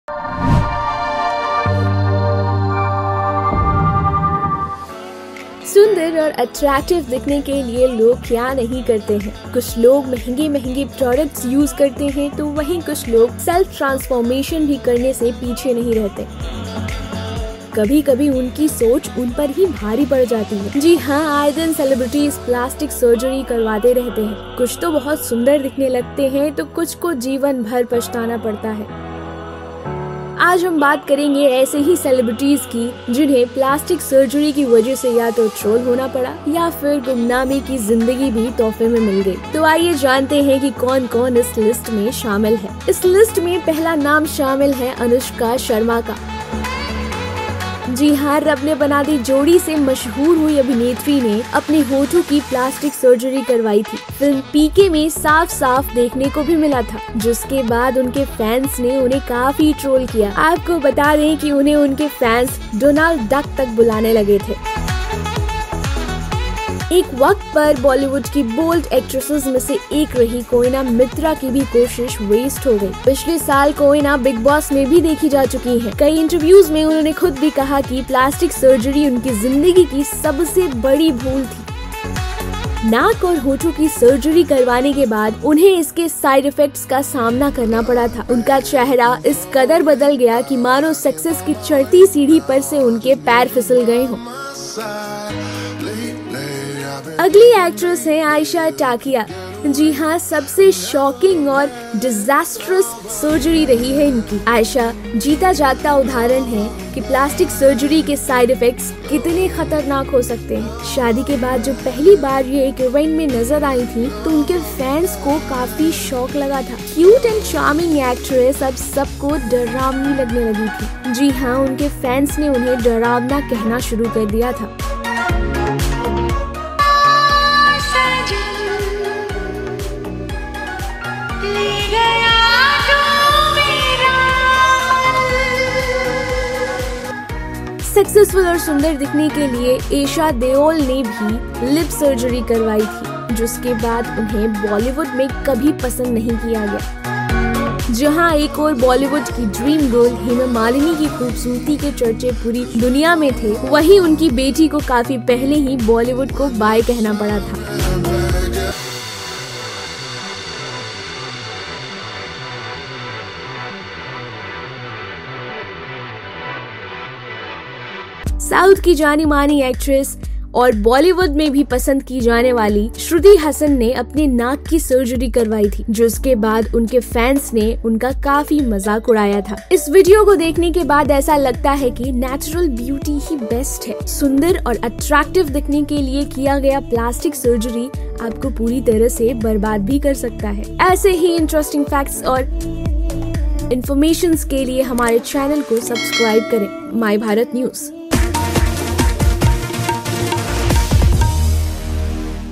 सर्जरी ने बिगाड़ा इन स्टार्स का लुक - Plastic Surgery से पहले और बाद की 10 बॉलीवुड अभिनेत्रियाँ
झाँसी, बाँदा, जालौन ,हमीरपुर, रामपुर, बस्ती, अयोध्या, अमेठी ,शाहजहांपुर मिर्जापुर जनपदों की बड़ी खबरें
बिहार चुनाव की हर ख़बर के लिए देखें ,